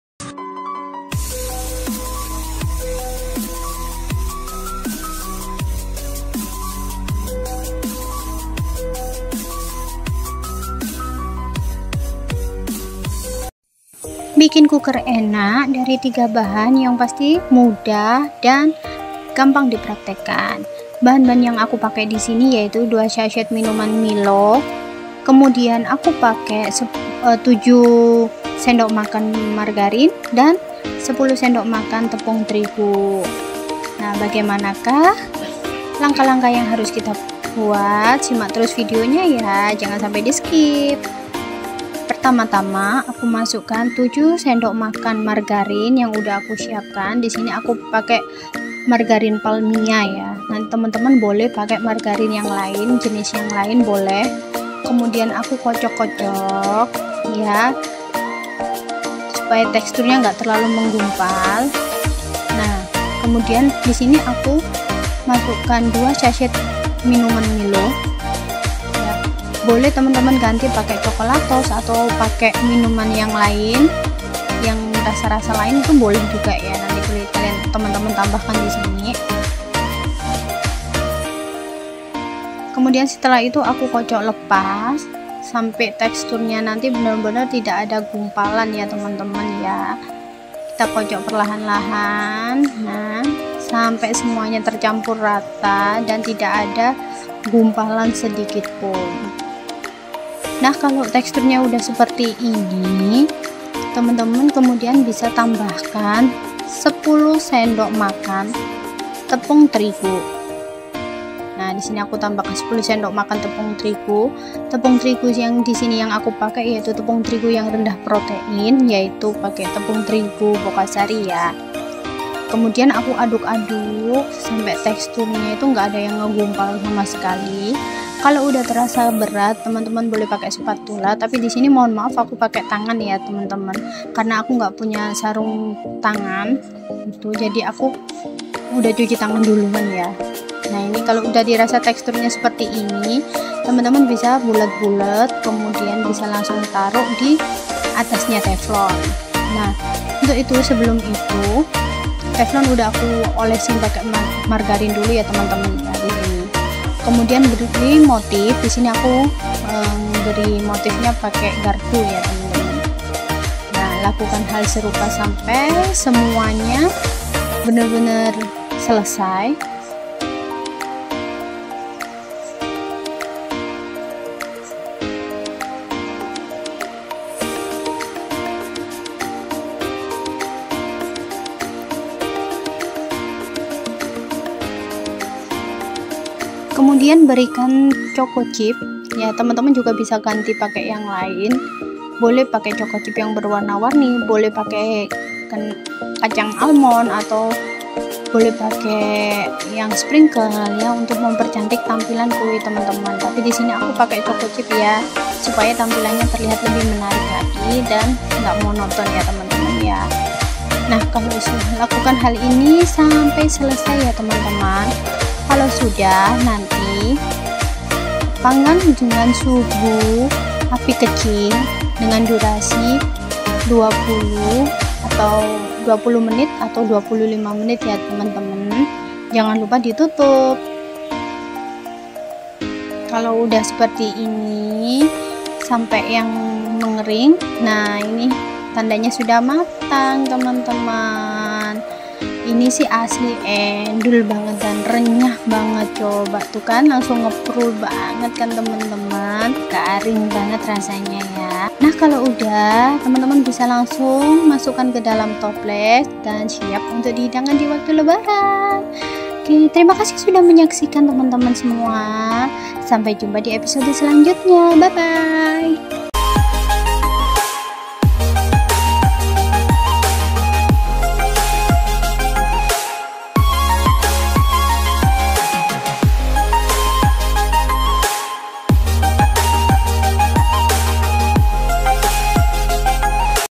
Resep Kue Kering 3 Bahan Anti Gagal |Tanpa Telur, Tanpa Oven , Tanpa Mixer
Resep kue kering dengan 3 Bahan. Mudah dan simpel banget buatnya. Pemula wajib coba nih..

Bahan-bahan :
Susu bubuk milo 2 sachet
7 sdm margarin
10 sdm tepung terigu protein sedang

Topping:
Chocochip

Disini aku pakai Oven kompor ya teman-teman. Sebelumnya telfon diberi tatakan terlebih dahulu supaya panasnya bisa merata.